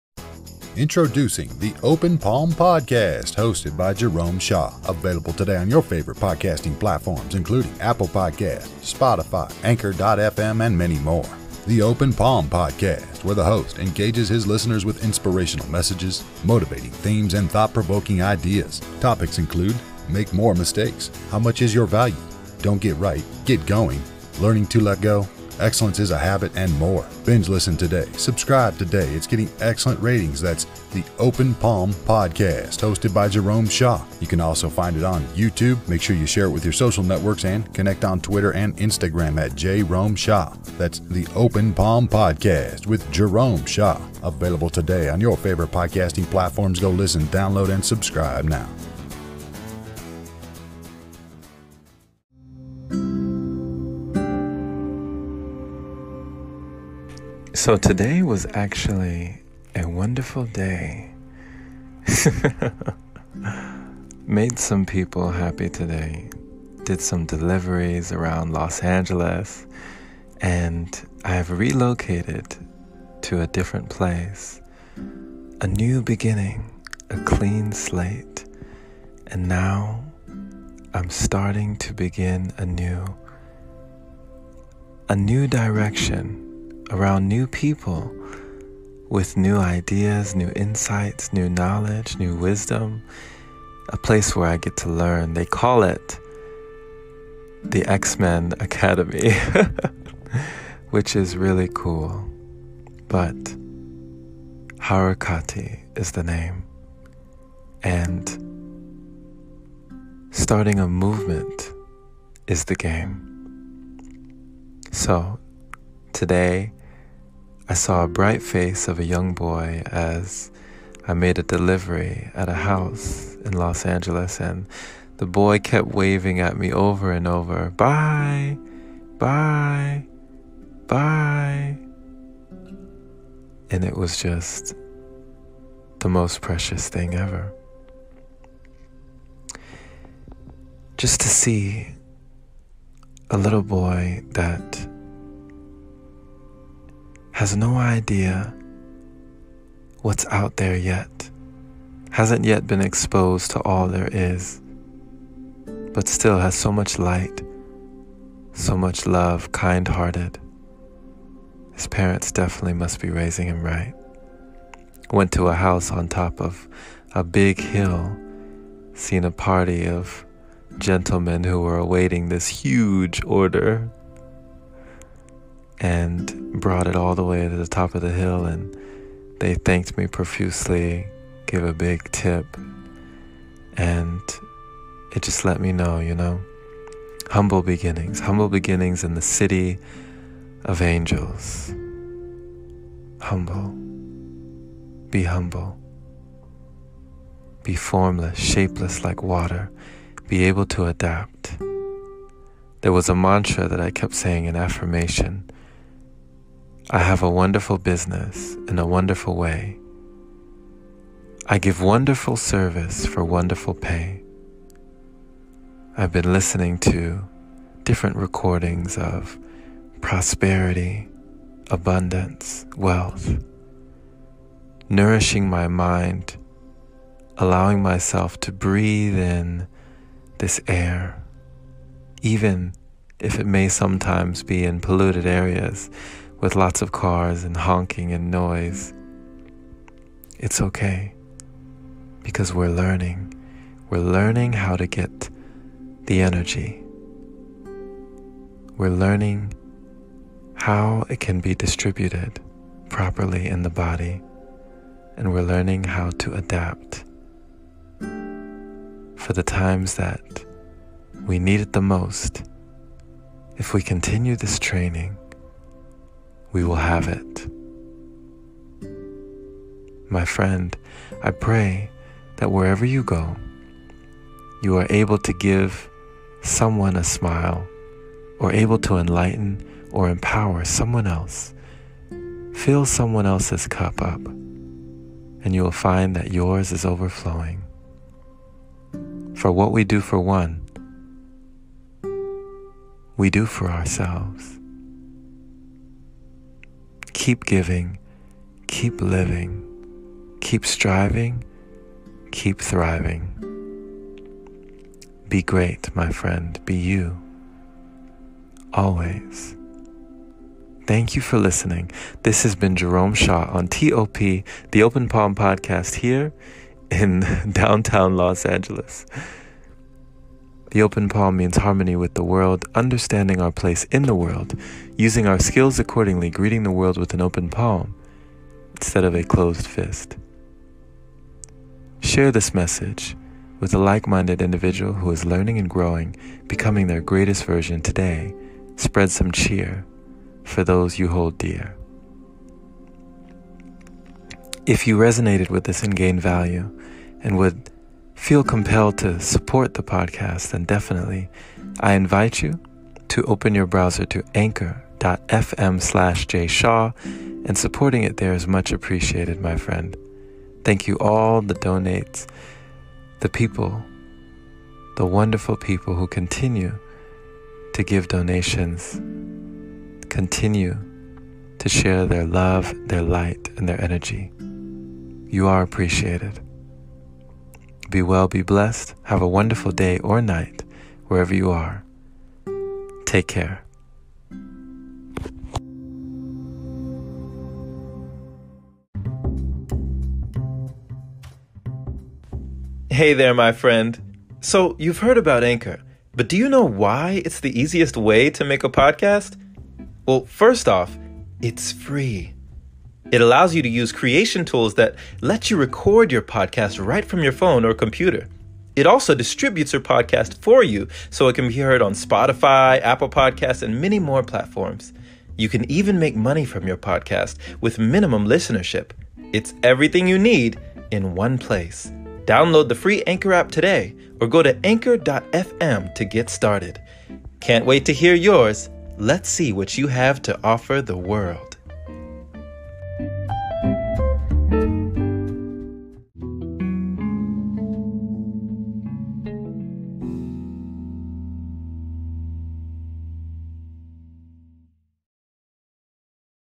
Spreading Cheer with No Fear 🤗
The Open Palm // New year, new slate. Many new beginnings are taking place. Excited to once again begin a journey. Today I enjoyed spreading cheer while making deliveries. Able to humble myself in knowing that I am part of all there is. You are as well my friend. You are a light that can spark many candles. Don’t wait till you have much to give. True living is a life of giving. You’ll find that you often receive even more than you give. Be fearless. Be blessed. Be you. GRAB THE NEW BOOK ON AMAZON!